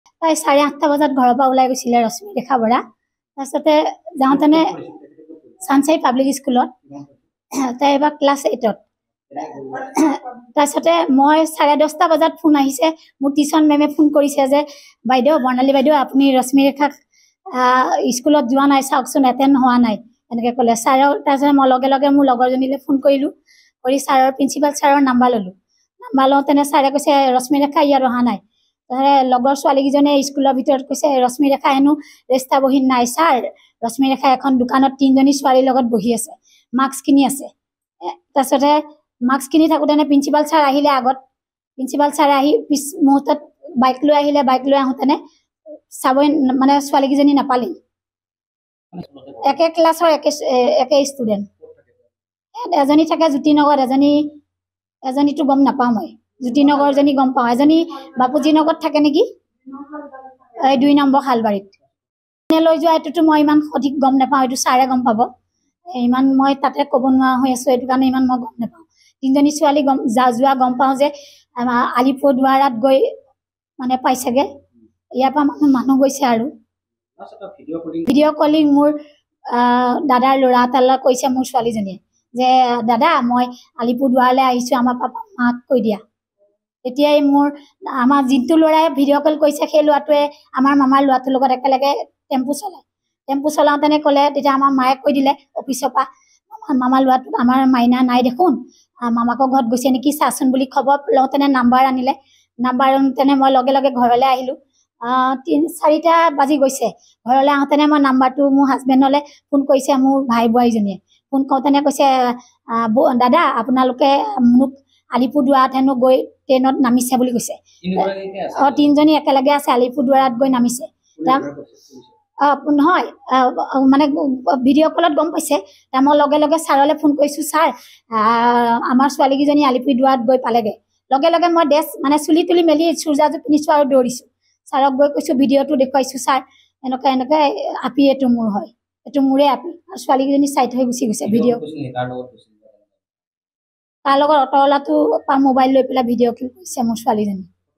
Guwahati |নাৰী সৰবৰাহকাৰীৰ কবলত গুৱাহাটীৰ কিশোৰী। নুনমাটিৰ তিনি কিশোৰী শিলিগুৰিত থকাৰ সন্দেহ | N18V
Guwahati |নাৰী সৰবৰাহকাৰীৰ কবলত গুৱাহাটীৰ কিশোৰী। নুনমাটিৰ তিনি কিশোৰী শিলিগুৰিত থকাৰ সন্দেহ | পৰিয়ালৰ মাজত হাহাকাৰ ।N18V